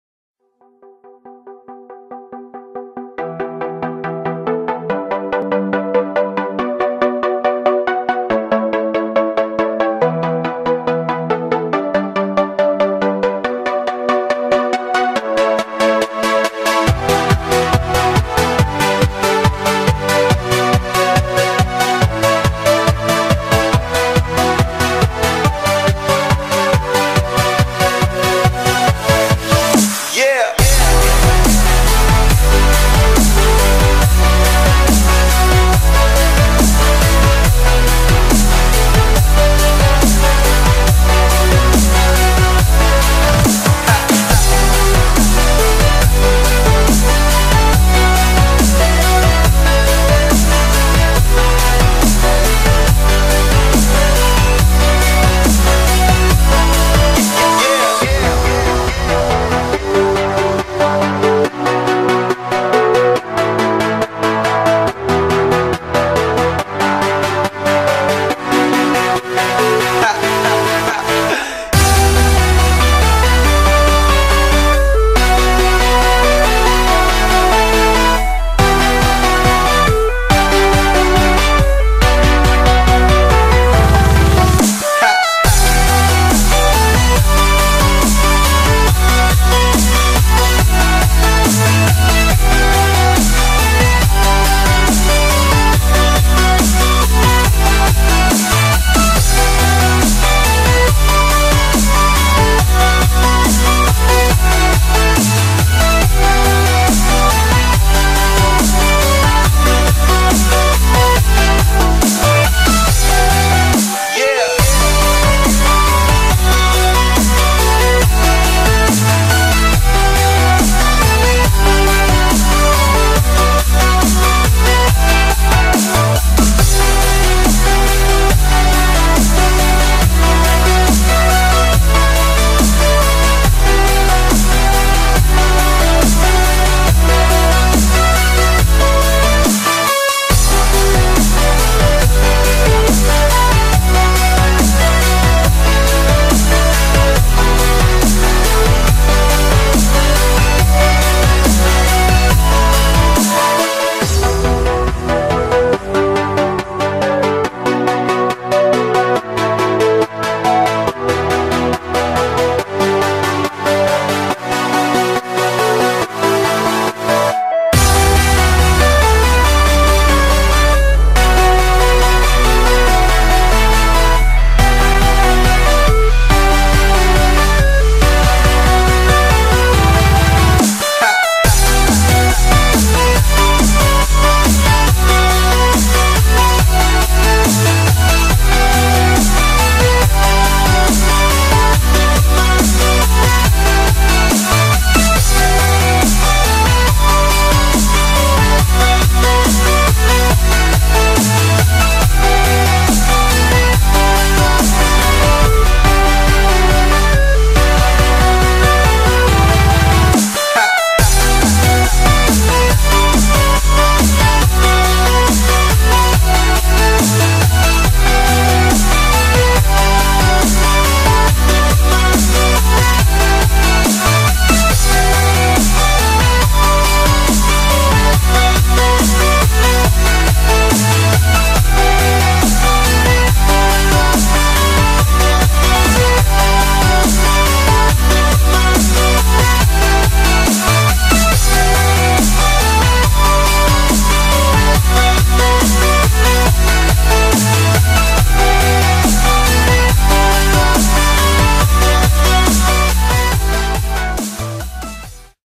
Bootcamp jsp
Bookcamp avec les JSP et encadrement de ma section dans l'Herault juin 2017.